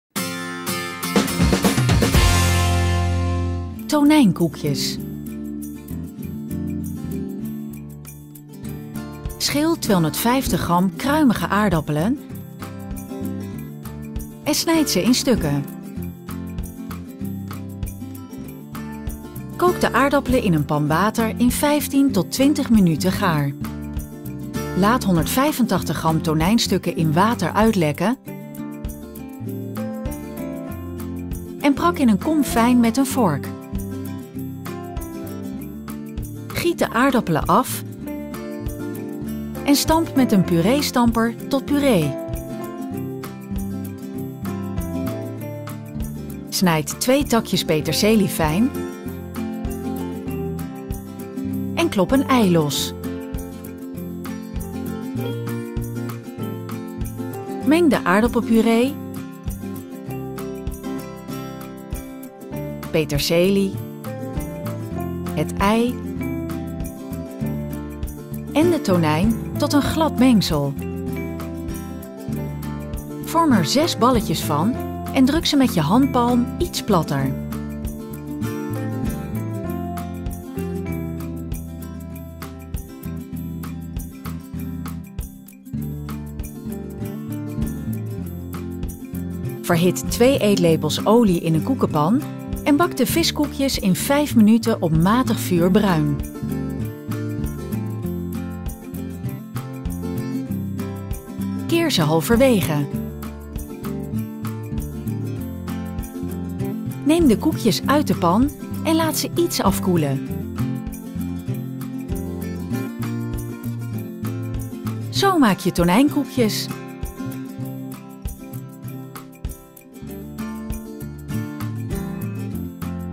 Opperdepop: tonijnkoekjes - Allerhande
Ben je op zoek naar een lekker recept voor je kindje? Dan biedt Allerhande uitkomst met deze lekkere tonijnkoekjes.

Aardappelen koken
Schil 250 gram kruimige aardappelen en snijd ze in stukken. Kook de aardappelen in een pan water in 15 tot 20 minuten gaar.

Tonijnmengsel
Laat 185 gram tonijnstukken in water uitlekken en prak in een kom fijn met een vork.
Giet de aardappelen af en stamp met een pureestamper tot puree. Snijd 2 takjes peterselie fijn en klop een ei los.
Meng de aardappelpuree, peterselie, het ei en de tonijn tot een glad mengsel. Vorm er 6 balletjes van en druk ze met je handpalm iets platter.

Tonijnkoekjes bakken
Verhit 2 eetlepels olie in een koekenpan en bak de viskoekjes in 5 minuten op matig vuur bruin. Keer ze halverwege.
Neem de koekjes uit de pan en laat ze iets afkoelen. Zo maak je tonijnkoekjes.